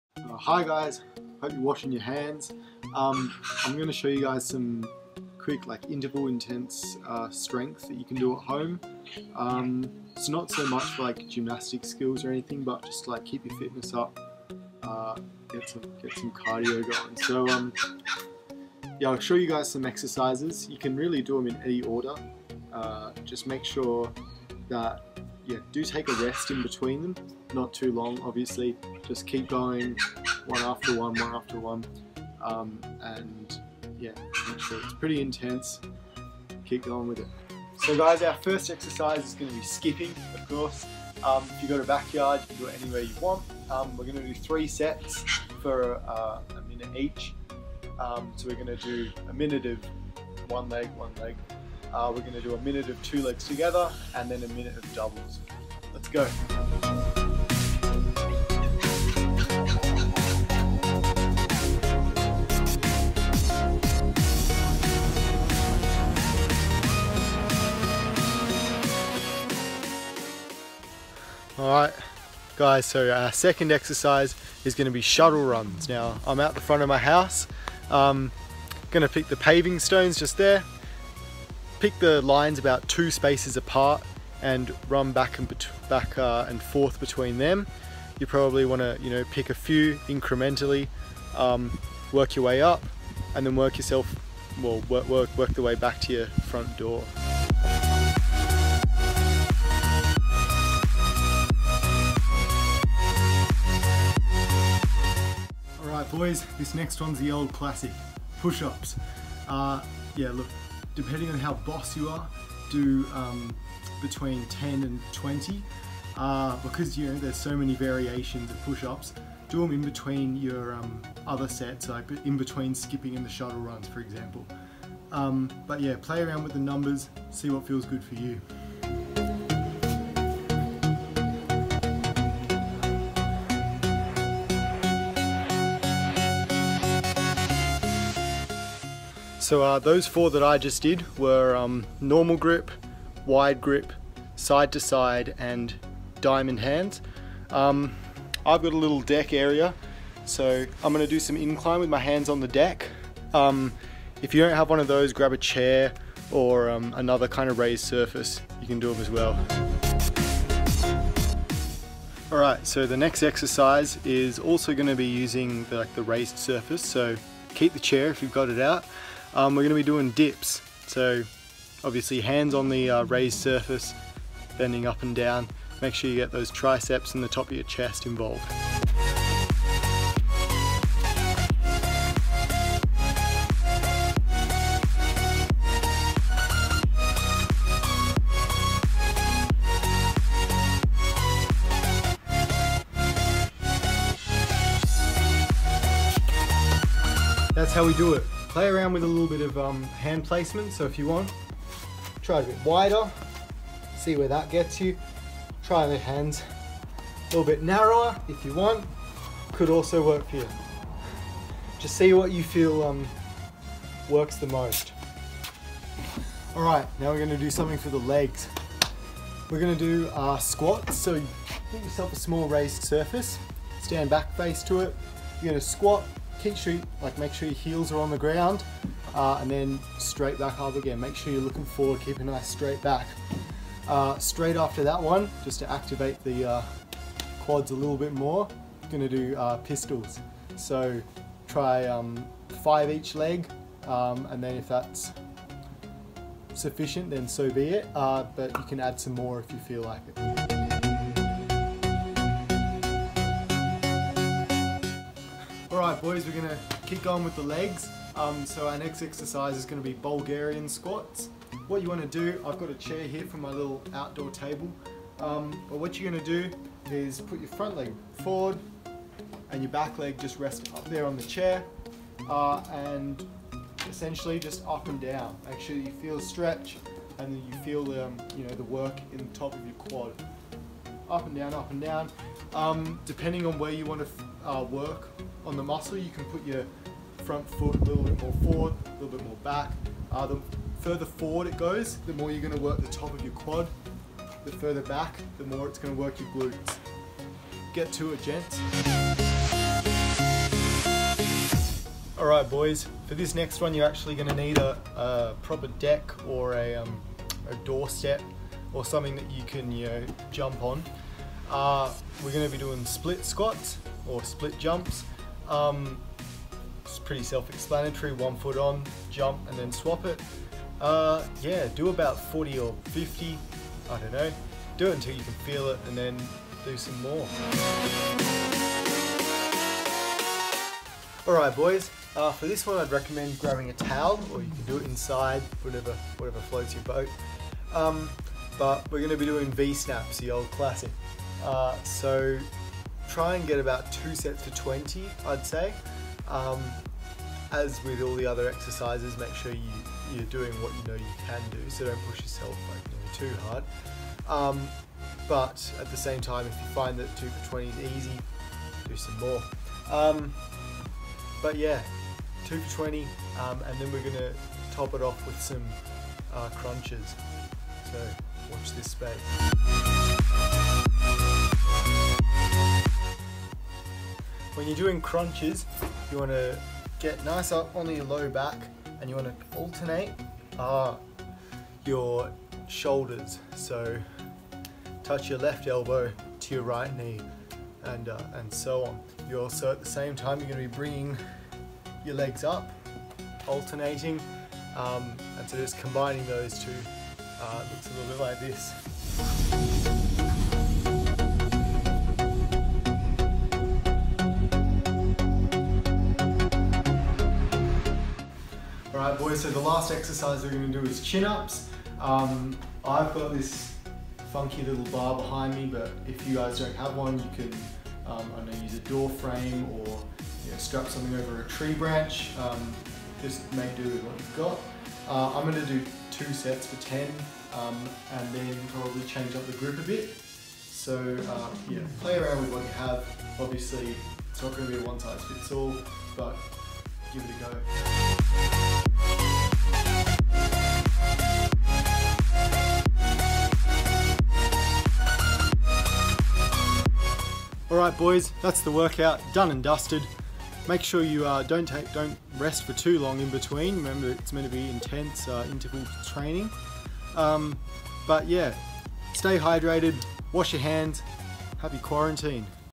Jack's at home strength workout
Canberra City Gymnastics Club senior MAG gymnast Jack gives us a great at home strength workout.
Follow along at home to help maintain or build your strength up!